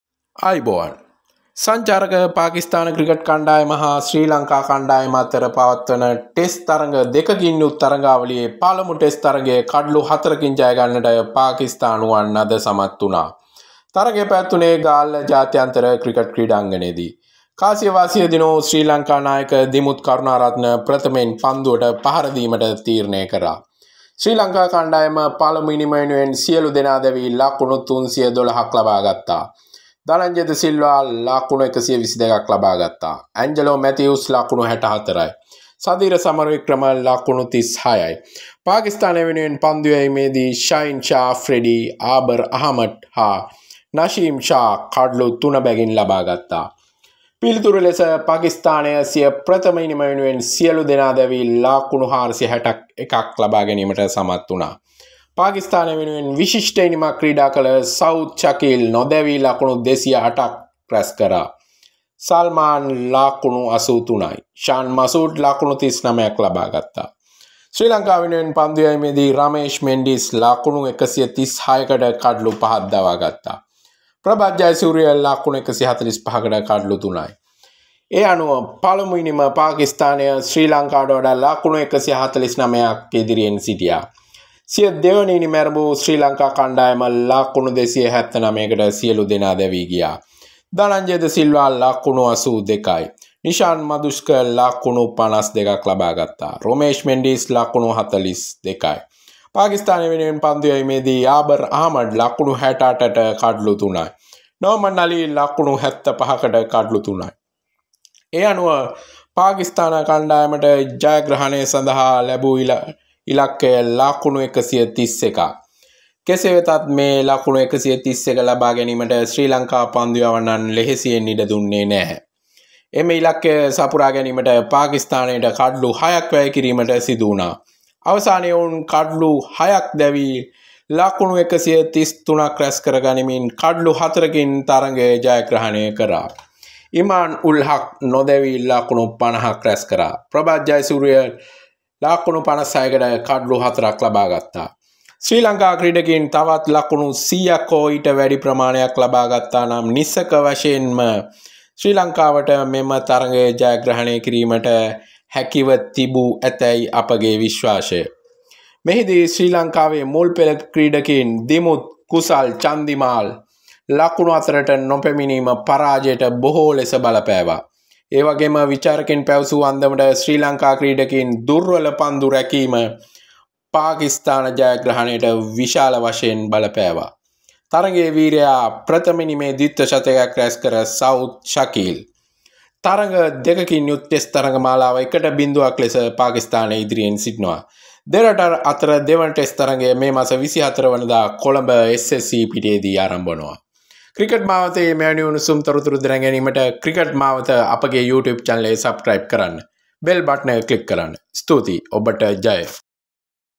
තව ලකුණු 100වත් වැඩිපුර ගැහුවනම් අපිට දිනන්න තිබුනා | pakistan tour of sri lanka 2023 | test match
test match | day 5 sri lanka vs pakistan
cricket mawatha